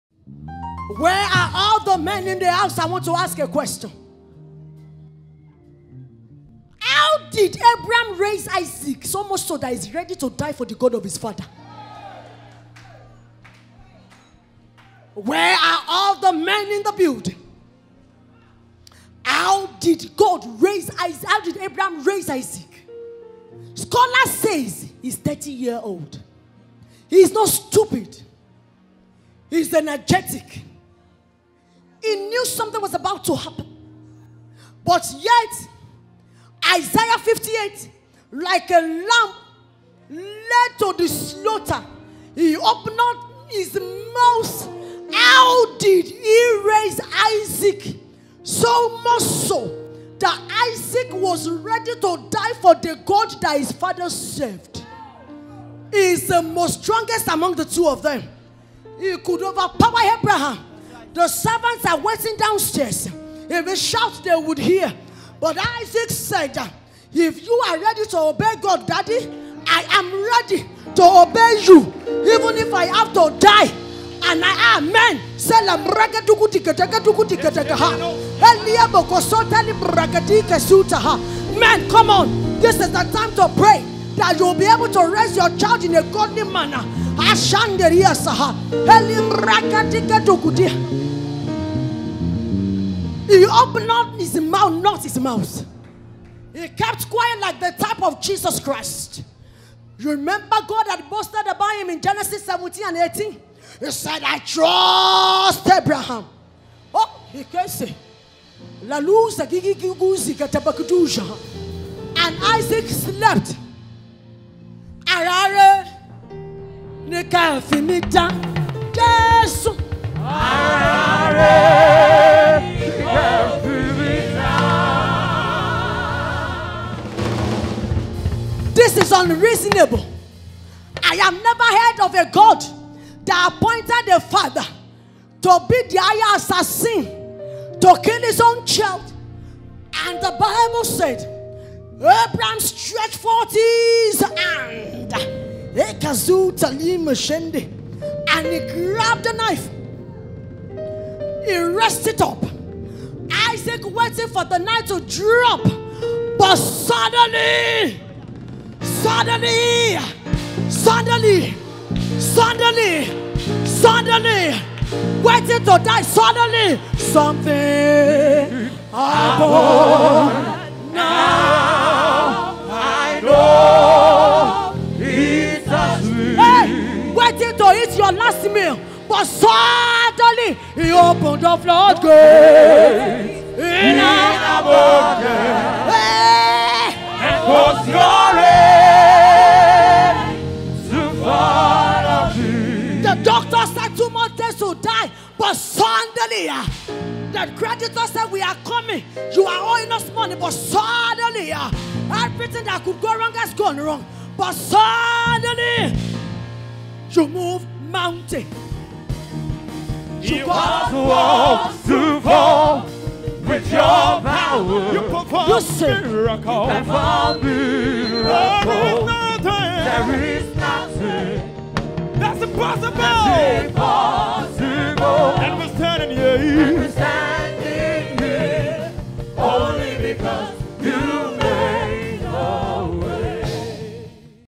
UNREASONABLE GOD MESSAGE 008
Sometimes God's dealings with your life does not make sense. What is God doing with my life? Can I at least understand what you're up to GOD?

An Inspiring Worship Drama, a story of Gods leading that makes no sense to man, AT THE BEGINNING.

WHERE ARE YOUR ACCUSERS?